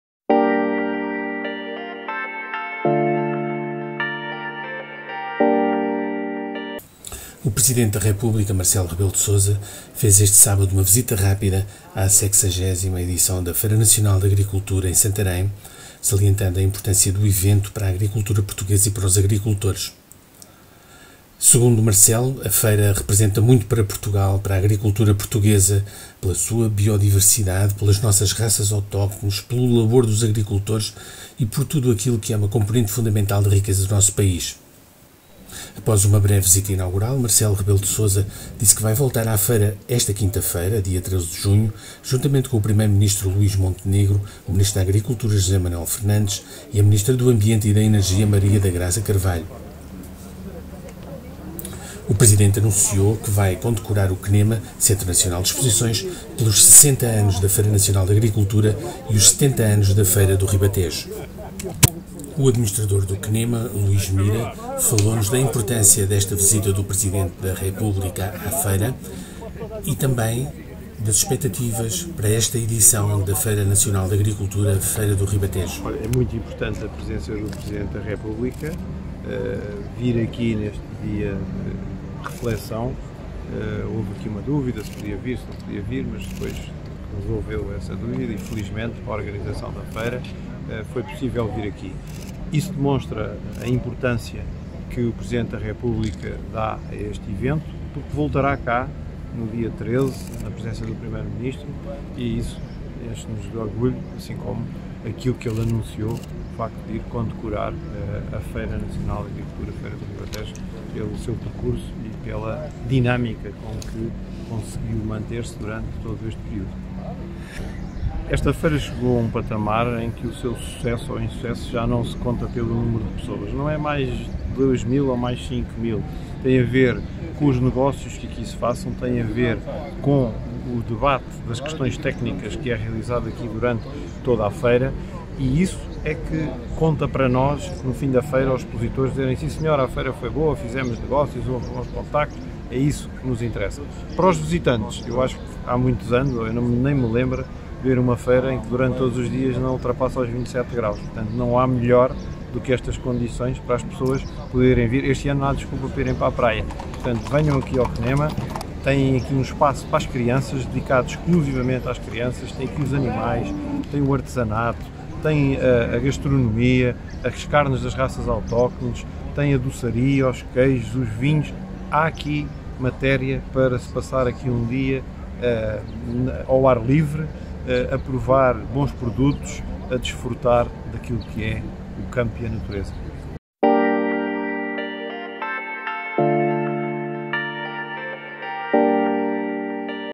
Marcelo visita Feira Nacional da Agricultura e destaca importância do setor
O Presidente da República quis marcar presença na inauguração da Feira Nacional de Agricultura, mesmo sendo dia de reflexão na véspera das Eleições Europeias. Por isso, optou por uma breve visita inaugural, e limitou as declarações para salientar a importância do certame para a “agricultura portuguesa” e para os agricultores.

A feira “representa muito por Portugal, pela agricultura portuguesa, pela nossa riqueza natural, pela nossa biodiversidade, pelas nossas raças autóctones, pelo labor pelos agricultores, por aquilo que é uma componente fundamental da riqueza do nosso país”, disse o chefe de Estado.
Apesar da breve visita, Marcelo Rebelo de Sousa prometeu voltar a visitar a feira nesta quinta-feira, dia 13 de junho, juntamente com o primeiro-ministro, Luís Montenegro, o ministro da Agricultura, José Manuel Fernandes, e a ministra do Ambiente e da Energia, Maria da Graça Carvalho.